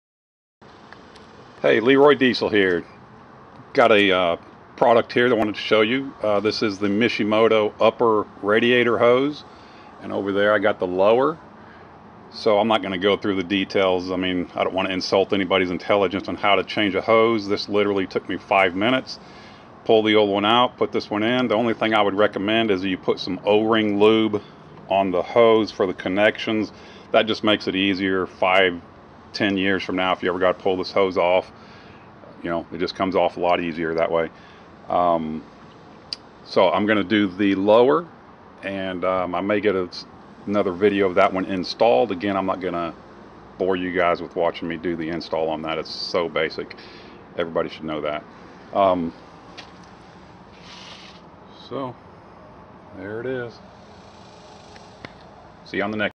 MishiMoto radiator hoses. Lifetime warranty!
Quick video of the silicone Misihmoto radiator hoses. Upper and lower. These are very high quality, have them installed on the shop truck as you see & they are extra heavy duty hoses. Should never have a problem with them or ever need to replace them again. Available in Red, Black, Blue color.